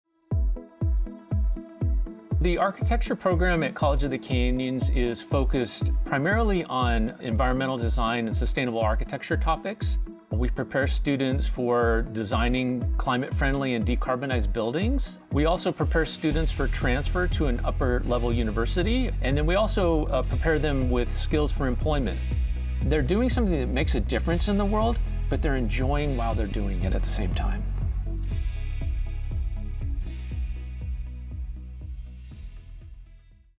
College of the Canyons Sustainable Architecture Program Video (Short)
College of the Canyons prepares students for careers in Architecture, Interior Design, and Sustainability as well as transfer to a 4-year/5-year program where they can earn a Bachelor's degree for their chosen major.

CreateEnergy.org
Canyons.edu

Short Video